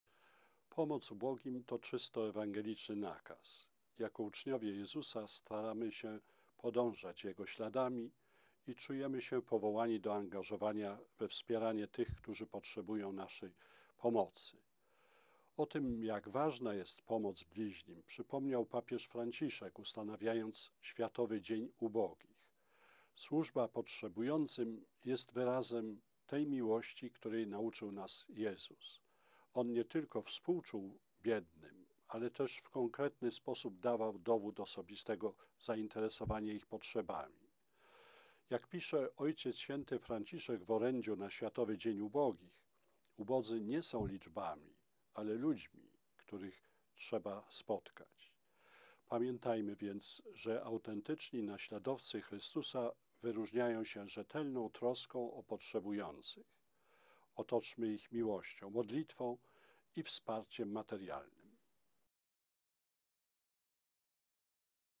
Przewodniczący KEP w Światowy Dzień Ubogich: Wspierajmy potrzebujących naszej pomocy
„Jako uczniowie Jezusa, staramy się podążać jego śladami i czujemy się powołani do angażowania we wspieranie tych, którzy potrzebują naszej pomocy” – podkreśla abp Stanisław Gądecki, Przewodniczący Konferencji Episkopatu Polski, w Światowy Dzień Ubogich.

Przewodniczący Episkopatu zaznaczył jednocześnie, że pomoc ubogim to czysto ewangeliczny nakaz. Przypomniał, że papież Franciszek, który ustanowił Światowy Dzień Ubogich w tegorocznym orędziu podkreślił, że „Ubodzy nie są liczbami, ale ludźmi, których należy spotkać”.

Abp Gądecki stwierdził: „Służba potrzebującym jest wyrazem tej miłości, której nauczył nas Jezus. On nie tylko współczuł biednym, ale też w konkretny sposób dawał dowód osobistego zainteresowania ich potrzebami”.

Przewodniczący Episkopatu podkreślił: „Pamiętajmy więc, że autentyczni naśladowcy Chrystusa wyróżniają się rzetelną troską o potrzebujących. Otoczmy ich miłością, modlitwą i wsparciem materialnym”.

Ustanowiony przez papieża Franciszka Światowy Dzień Ubogich w tym roku będzie obchodzony będzie po raz trzeci – w niedzielę 17 listopada. Jego mottem są słowa Psalmu 9 „Ufność nieszczęśliwych nigdy ich nie zawiedzie”.

BP KEP